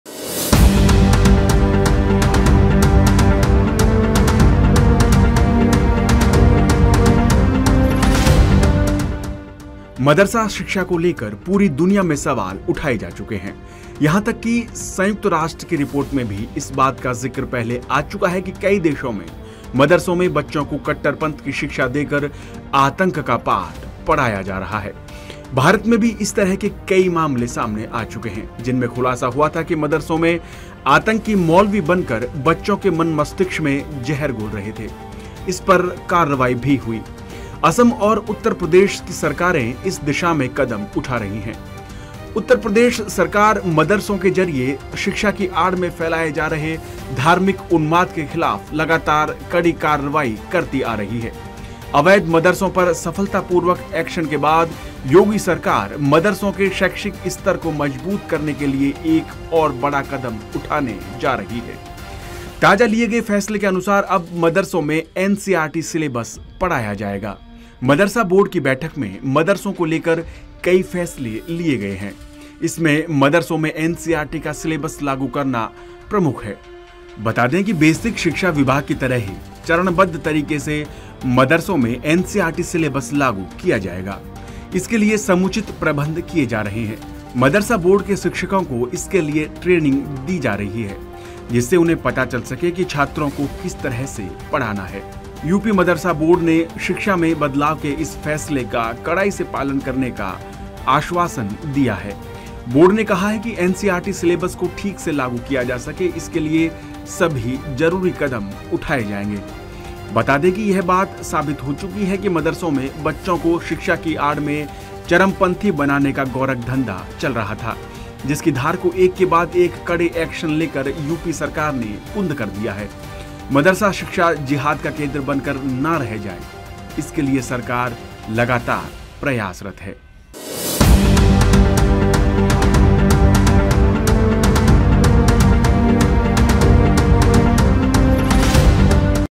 अब UP के मदरसों में NCERT की किताबों से होगी पढ़ाई | Yogi Govt | Madrasa
Description-
उत्तर प्रदेश सरकार मदरसों के जरिए शिक्षा की आड़ में फैलाये जा रहे धार्मिक उन्माद के खिलाफ लगातार कड़ी कार्रवाई करती आ रही है। अवैध मदरसों पर सफलता पूर्वक एक्शन के बाद योगी सरकार मदरसों के शैक्षिक स्तर को मजबूत करने के लिए एक और बड़ा कदम उठाने जा रही है। ताजा लिए गए फैसले के अनुसार अब मदरसों में एनसीआरटी सिलेबस पढ़ाया जाएगा।